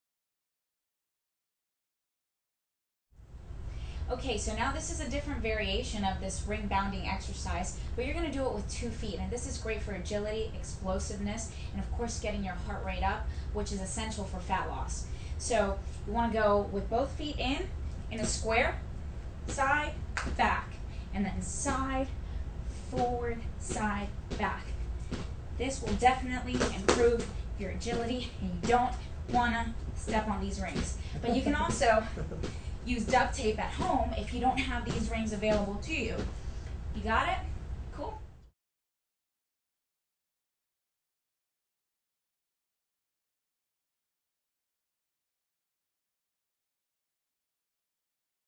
PACE Fitness Exercise - Bounding with Rings Variation - Plus FREE PACE eBook
Certified PACE Instructor, Coach Yari demonstrates a variation on Bounding with Rings.

Find out why PACE Express - The 12-Minute Fitness Revolution is so much better than the fitness programs the "cardio experts" recommend.